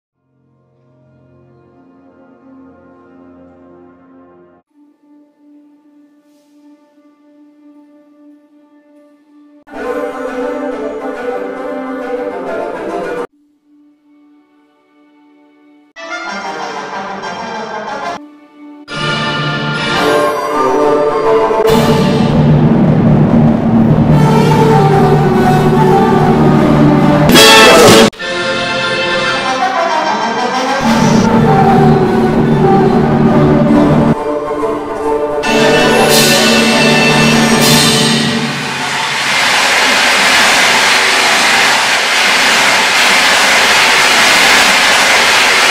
Happy Halloween! From the Jacksonville Symphony Orchestra
How terrifying is it to play in the Jacksonville Symphony wind section? We dare you to find out! Thanks to all the musicians who captured these shots.